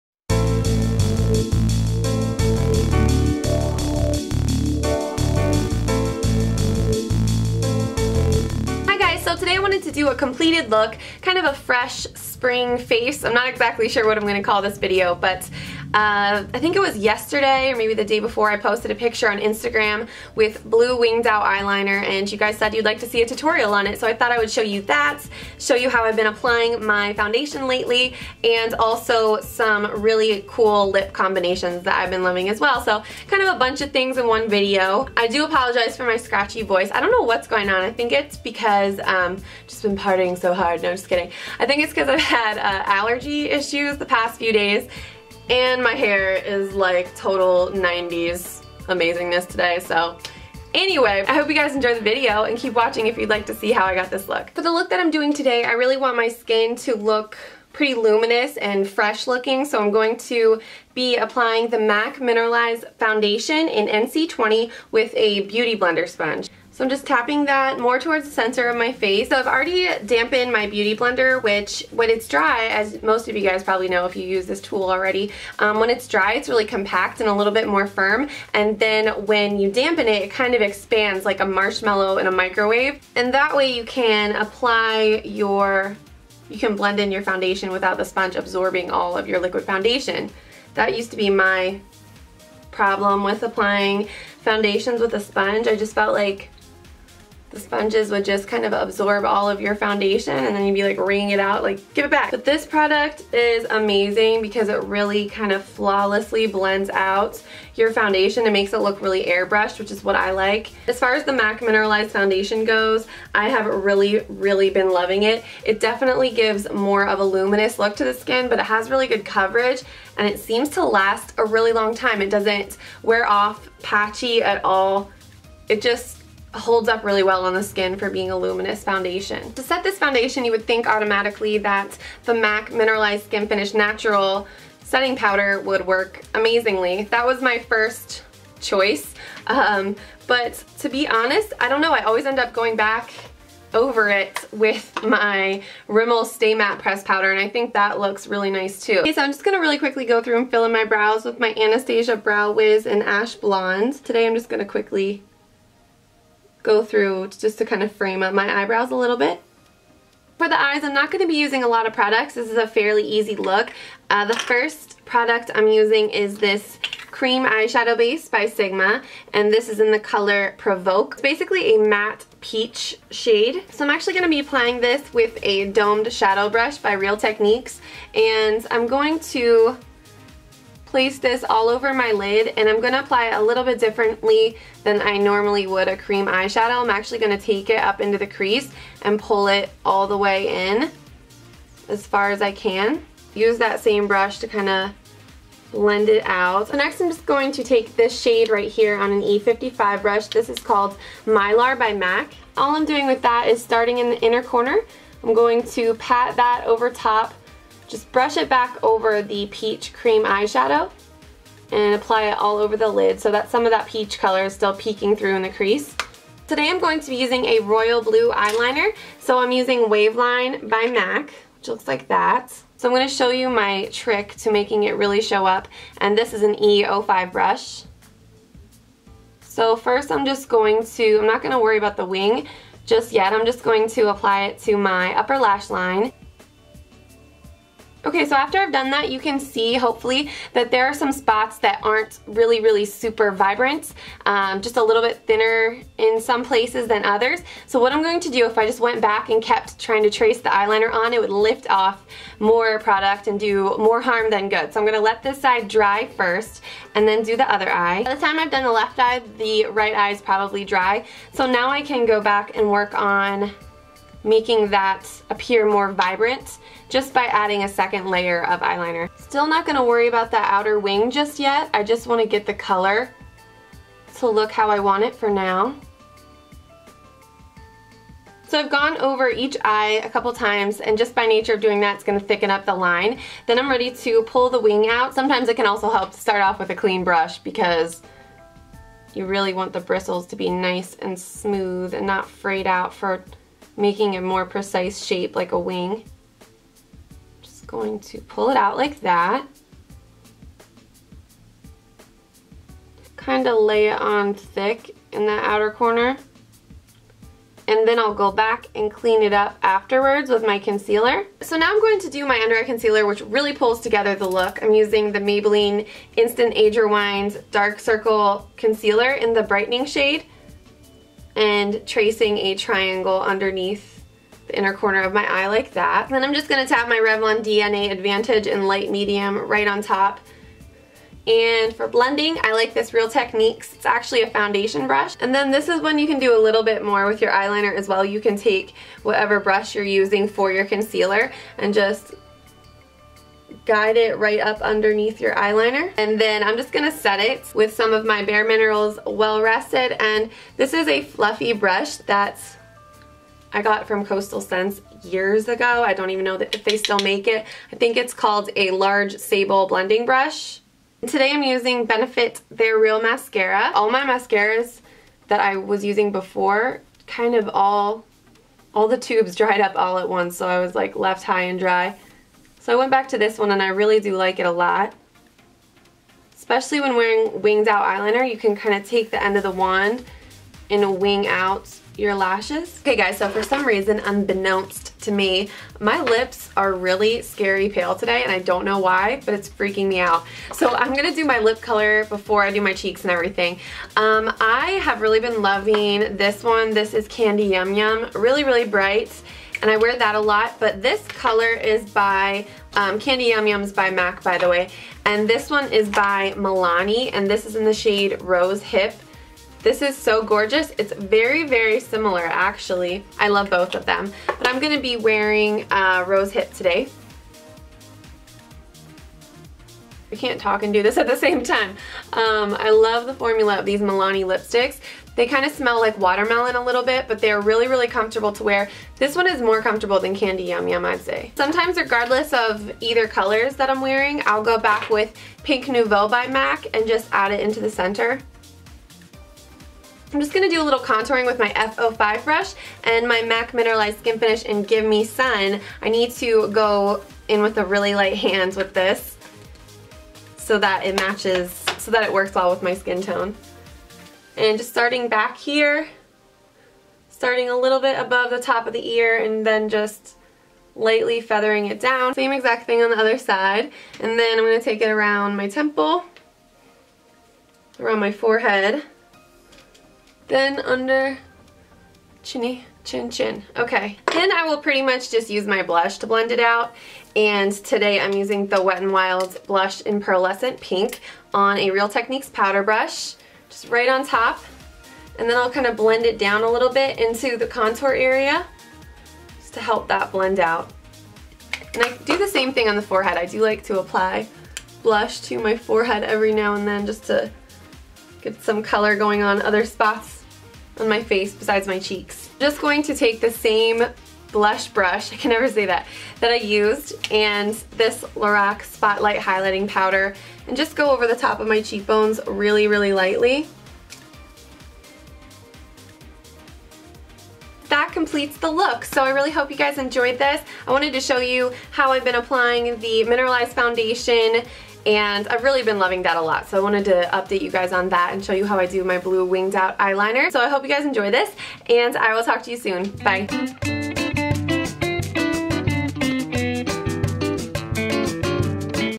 Complete Look ♥ Blue Winged Liner + Bright Lips
Thanks for requesting this look via Instagram!  @makeupbyalli

Products Used:
Beauty Blender
MAC Mineralize Moisture foundation NC20 (new product)
MAC Mineralize Skinfinish Natural, Medium Plus
Rimmel Stay Matte powder, Nude Beige
Anastasia Brow Wiz, ash blonde
Sigma Provoke eyeshadow base
MAC Mylar eyeshadow
MAC Waveline Fluidline
Maybelline Age Rewind concealer, "Brightening"
Revlon DNA Advantage concealer, Light/Medium
Bare Minerals Well Rested powder
Benefit They're Real mascara
Milani Rose Hip lipstick
MAC Pink Nouveau lipstick
MAC Mineralize Skinfinish, Give Me Sun
Wet N Wild Pearlescent Pink blush
Lorac Spotlight Highlighting powder

What I'm Wearing:
top: Love Culture
Hoops: Charming Charlie
Rings: Etsy
           Nail Gems are the "Flirty" brand (?) from CVS

Software: Adobe Premiere Pro and After Effects